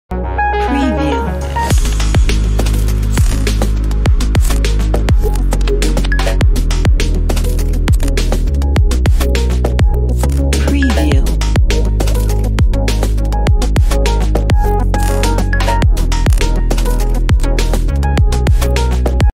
Store Typography Stories After Effects Templates
Store Typography Stories is a new After Effects template that pulls together a stunning combination of effects to make up 4 trendy looking vertical designs. They're so easy to use, simply edit the text, adjust the colors and hit render. You can use them on your Instagram account. Impress your audience with this unusual looking and eccentrically animated AE template. No plugins required.